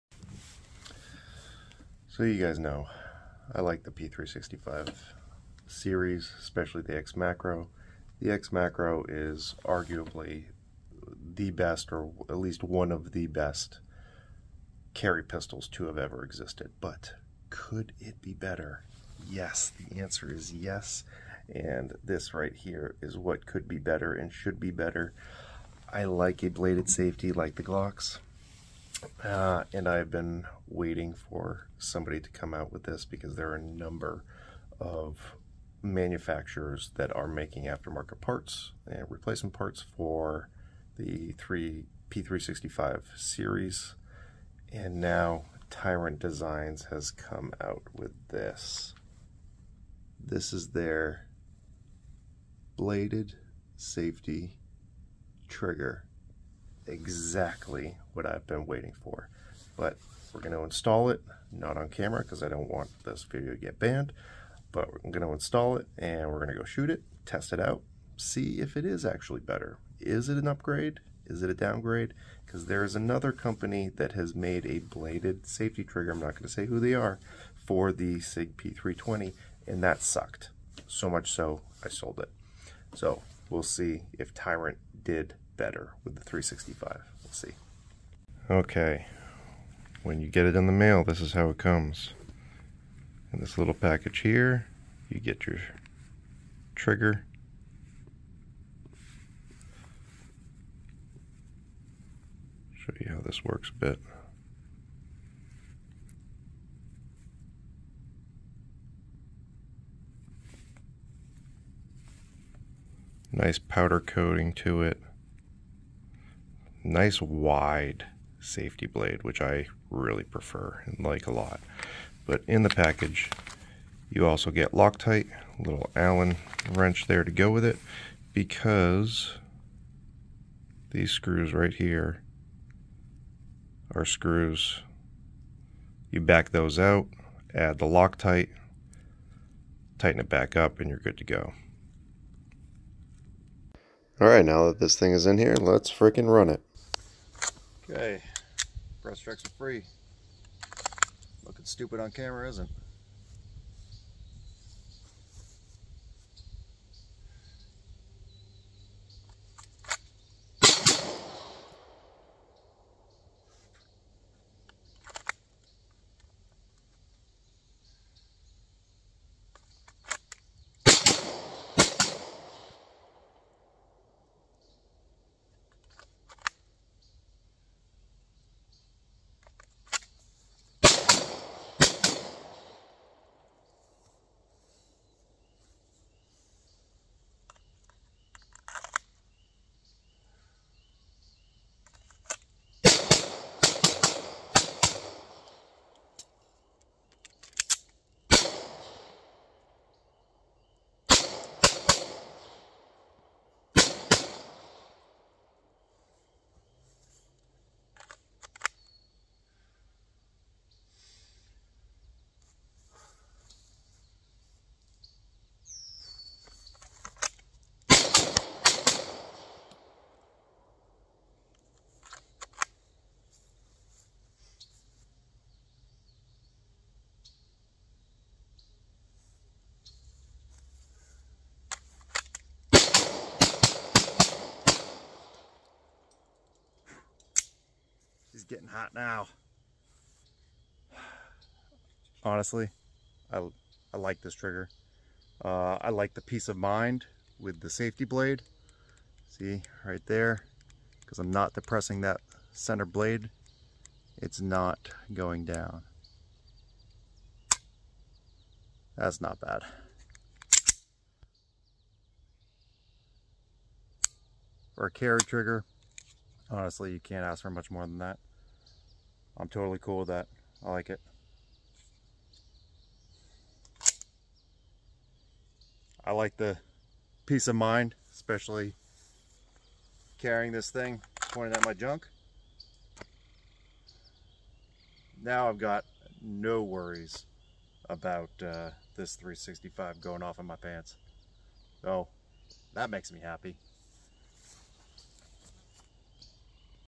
Tyrant CNC Sig P365 Intellifire Trigger
Quick look at and initial thoughts on the new blade safety trigher by Tyrant CNC for the Sig P365 series.